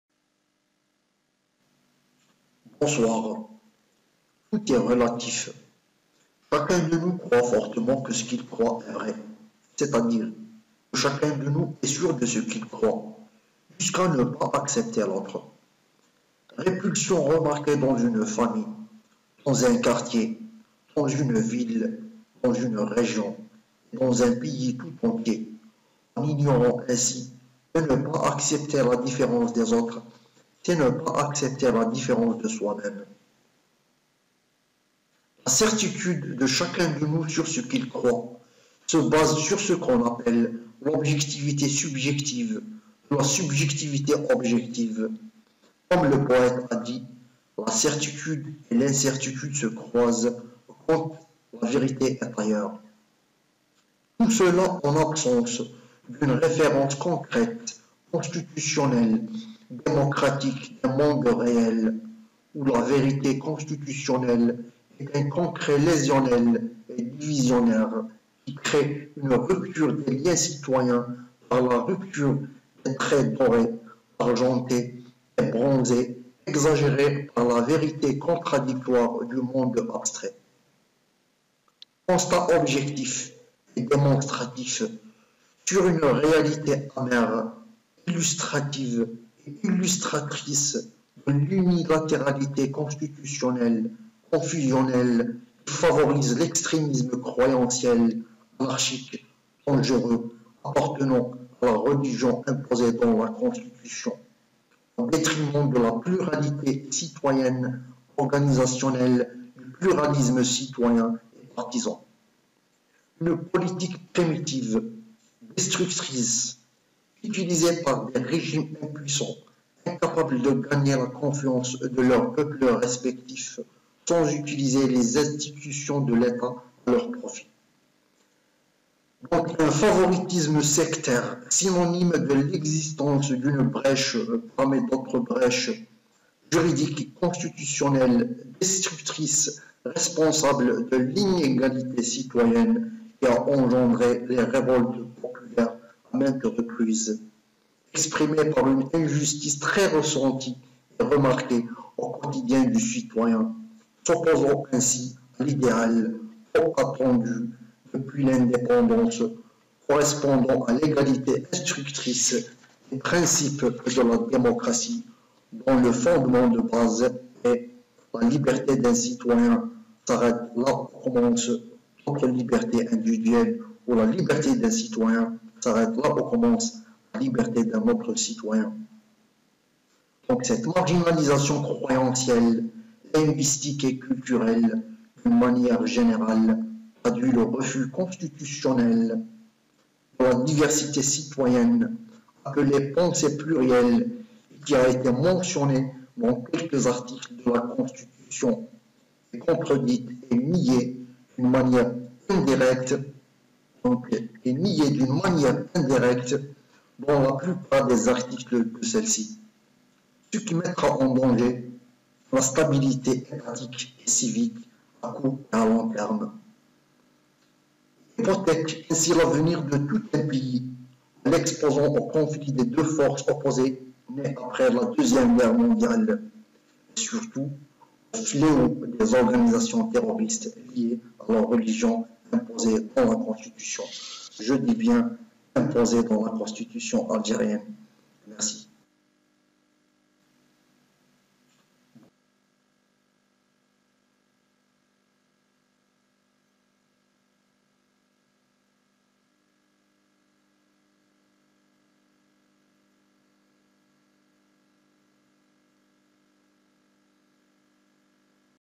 j'ai ecris cet article intitulè " la constitution " en 2001
smail moulai qu'on surnomme " le poète berbère dechu " , Rue de la libertè près du cinema 15004  mirabeau  wilaya de tizi-ouzou grande kabylie Algerie